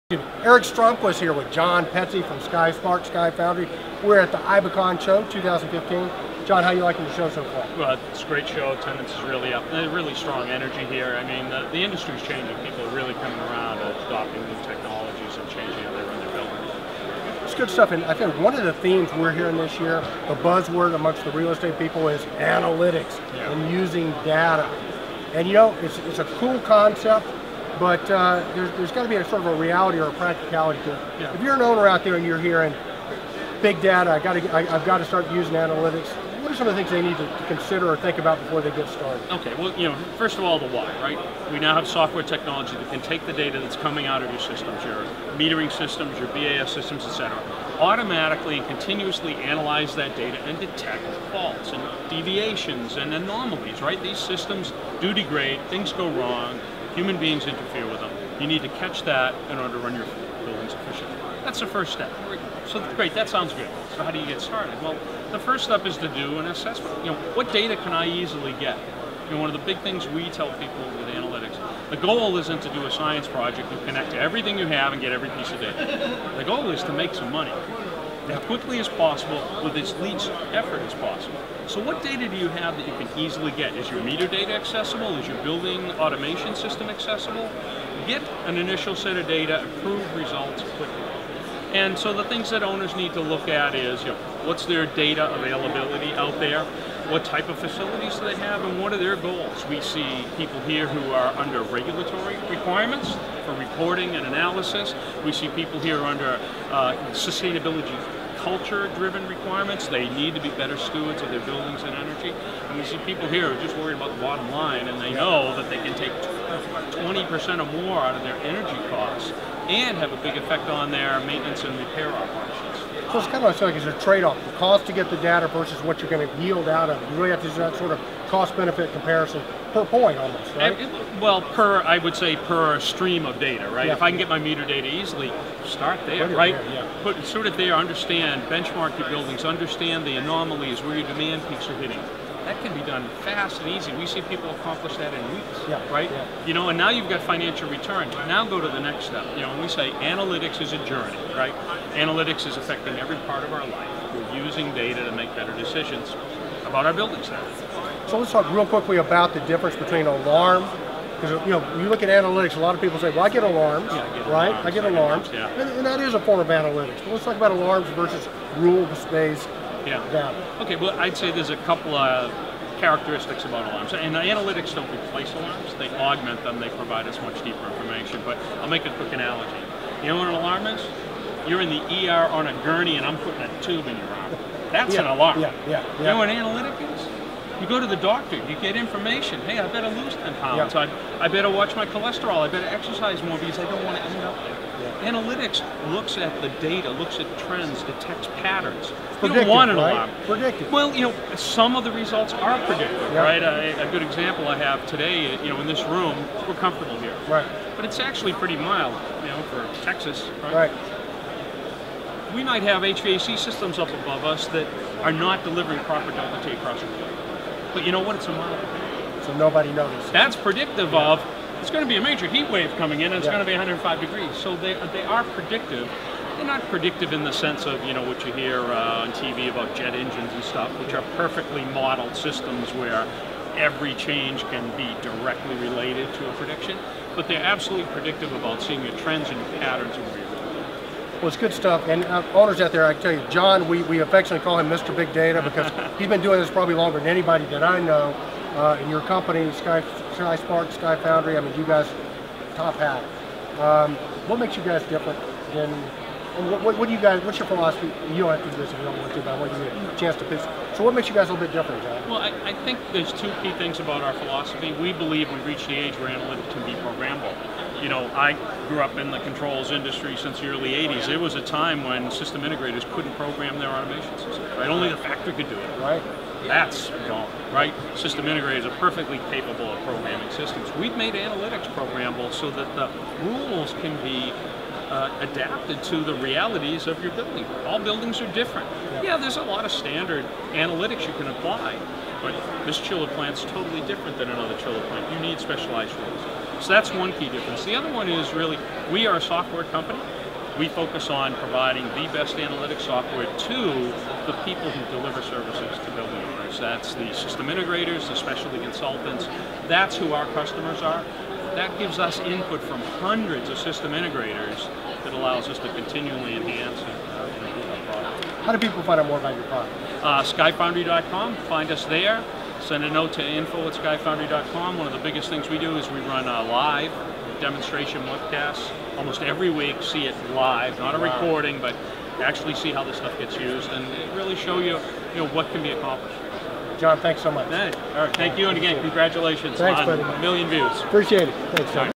John Petze of SkyFoundry talks with Eric about Building Analytics
John tells us why alarming is good but just a small piece of analytics and how SkySpark can give you all of the building data you need.